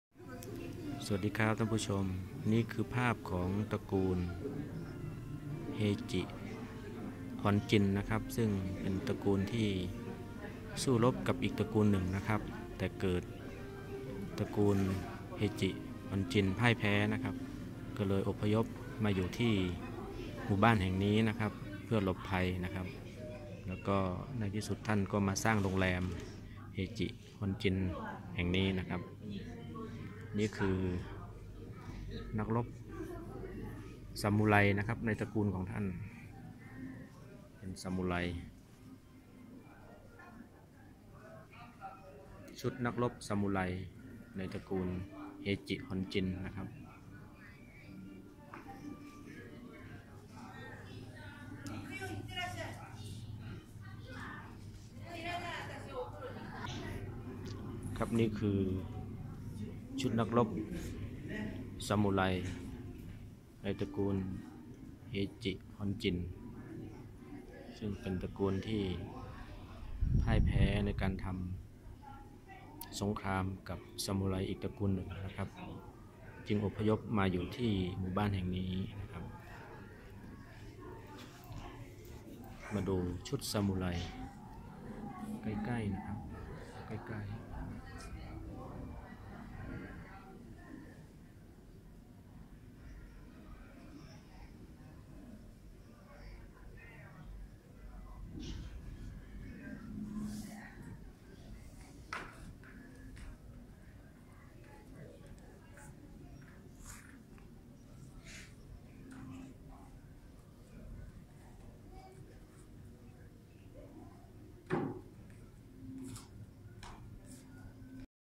คนดอยเที่ยวหิมะ/ตอนประวัติโรงแรม Heike honjin Yunishigawa Nikko Japan
ชมประวัติ ที่มาของโรงแรม Heike honjin Yunishigawa Nikko Japan